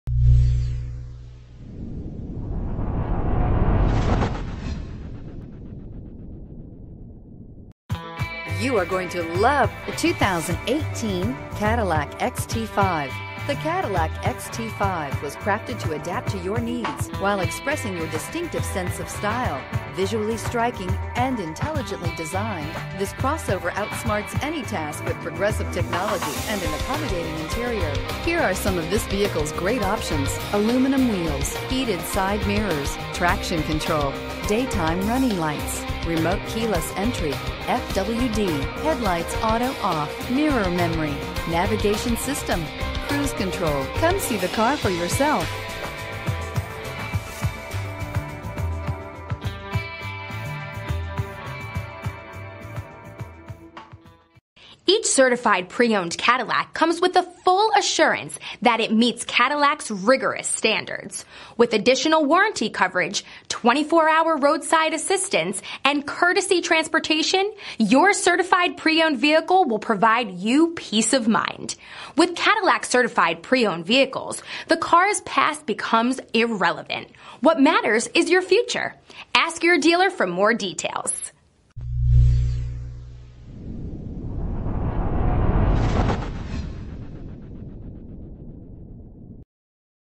2018 Cadillac XT5 Used Rusty Wallace Auto Morristown, TN Live P1992A
Certified. dark granite metallic 2018 Cadillac XT5 Premium Luxury FWD 8-Speed Automatic 3.6L V6 DI VVT BLUETOOTH HANDSFREE, 155 Amp Alternator, Body-Color Heated Power-Adjustable Outside Mirrors, Exterior Parking Camera Rear, Front & Rear Park Assist, Low Speed Forward Automatic Braking, Navigation System, Power Liftgate, Power moonroof, Premium Luxury Package 1SE, Steering wheel mounted audio controls, Ventilated Driver & Front Passenger Seats, Ventilated front seats, Wheels: 20 x 8 Sterling Silver Painted Finish.brbrCadillac Certified Pre-Owned Details:brbr  * Vehicle Historybr  * Warranty Deductible: $50br  * Limited Warranty: 72 Month/100,000 Mile (whichever comes first) from original in-service datebr  * 172 Point Inspectionbr  * Roadside Assistancebr  * Transferable WarrantybrbrbrRecent Arrival! 19/26 City/Highway MPG